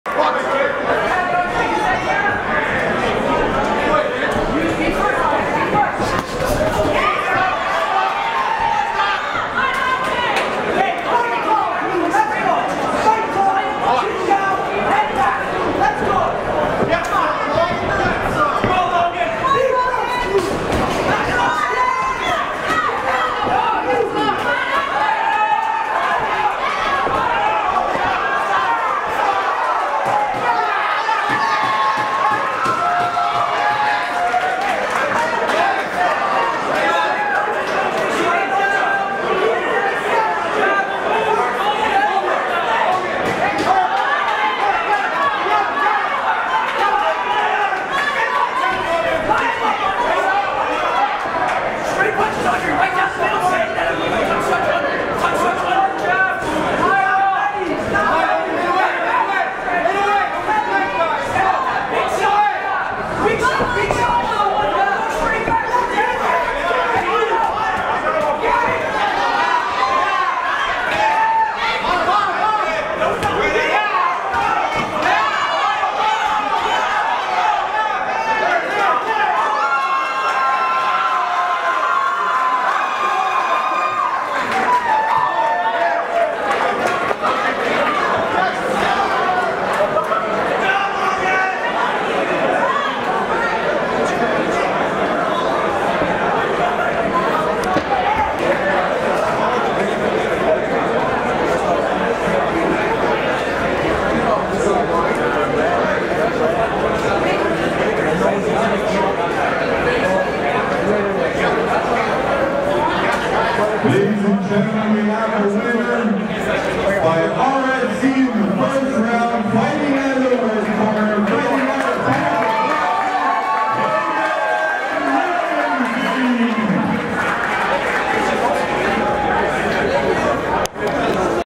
LOGAN LINDSAY VS ANDREI MELNIKOV AT HUF GYM ON SEPT 7
LOGAN LINDSAY PINNACLE BOXING
 VS
ANDREI MELNIKOV KINGSMAN BOXING